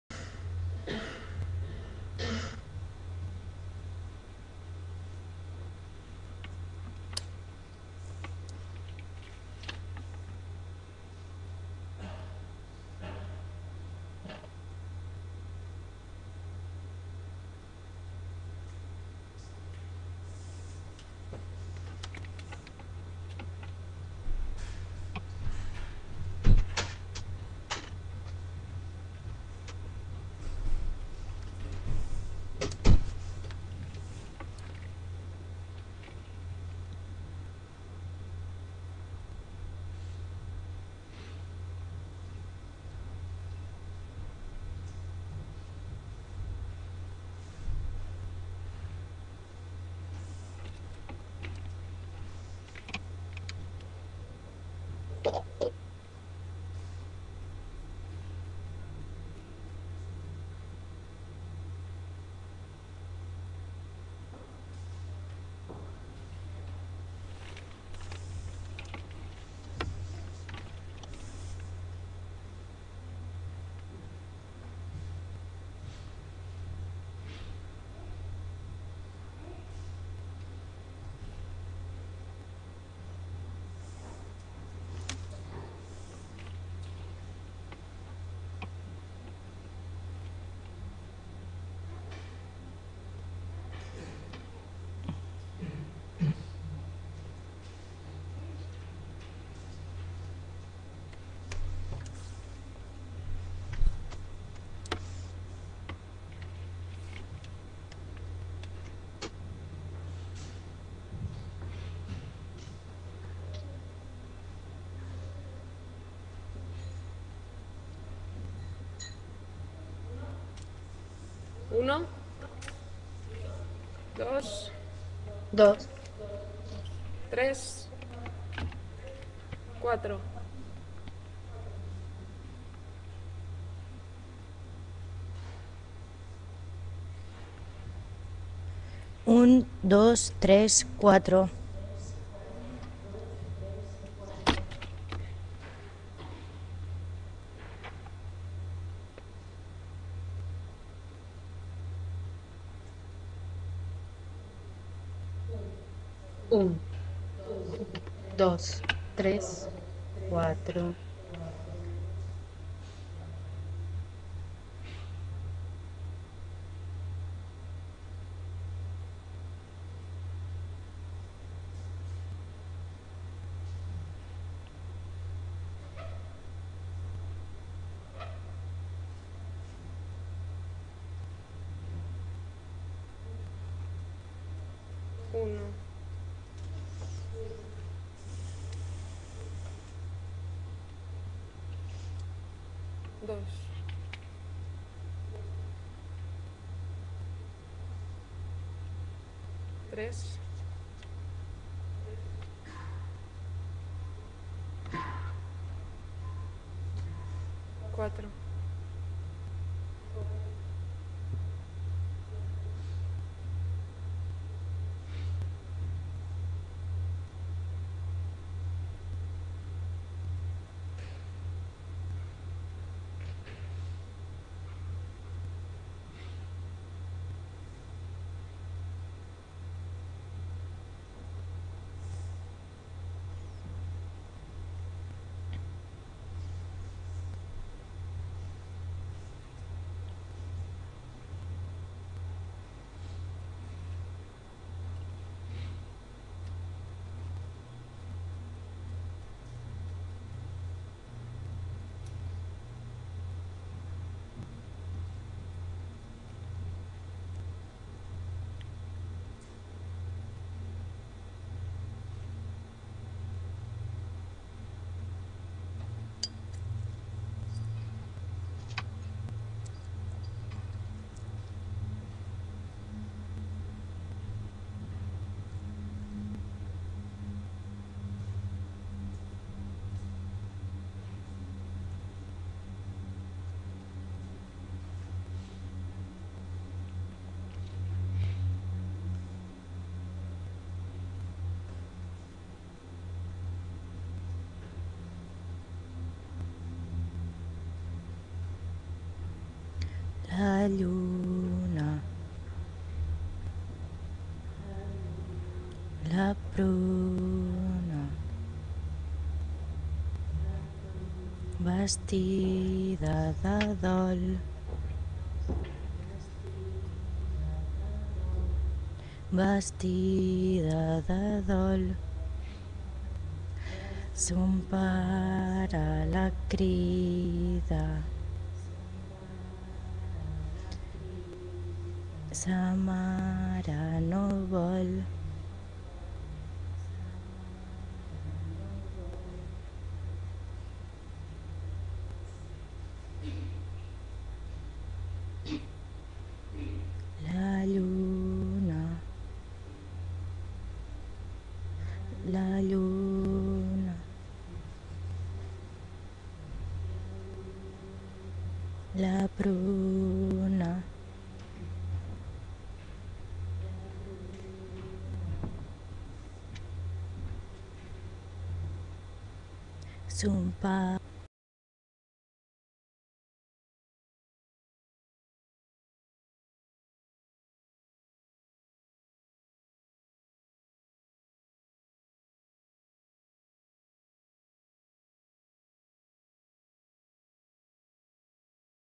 una dos tres cuatro
Study for a protocol for Huis Clos / No Exit performance series by Annie Abrahams.
21 Nov. 2008, L’animal a l’esquena, Celra, Spain. International Laboratory "Interactive digital media on stage" Organisation NU2's.
Technical assistance: Clément Charmet.
Thanks to the dancers Ana Sanchez Couso, Helena Pellisé and Maria Claudia Mejia. Information